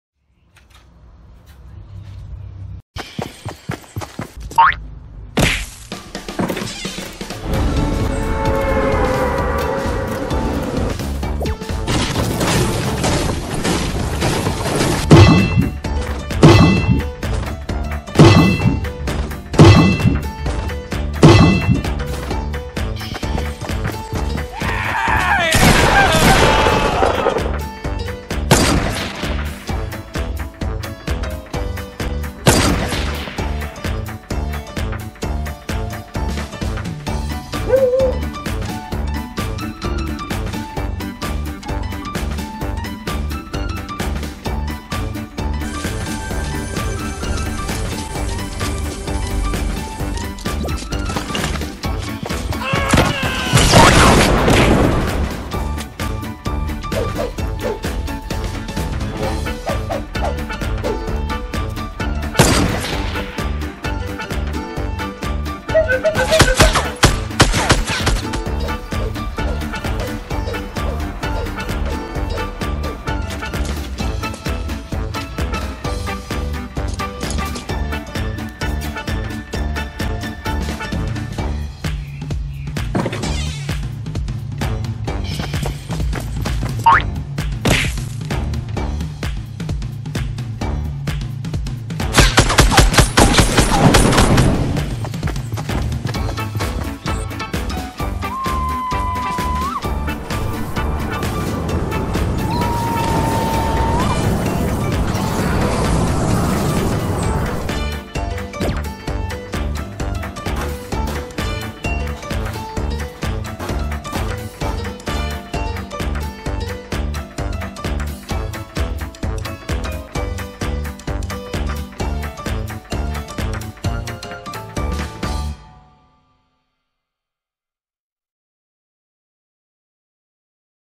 Fortnite Dances in Real Life Animation | Funny Fortnite Compilation
Fortnite Dances in Real Life Animation | Funny Fortnite | Surprise for Kids
My dogs having a dance party with alot of Fortnite skins. Maybe we can invite Ninja some time to. These Fortnite characters had a downtown drop in our city looking for Fortbytes i think but the only thing they get is a Fortnite storm flip for dinner!
They are jumping behind the fence and getting blown away by some wind turbines.
We have alot of dances in this video but the best of the best , and i mean the glitter emote is not in it , sorry lol.

Make sure you watch our Despicable ME 4 animation in real life videos with all the minions dancing , singing and having fun!